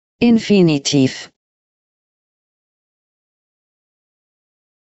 Infinitiv
This is the Microsoft Hedda voice that is reading the word infinitiv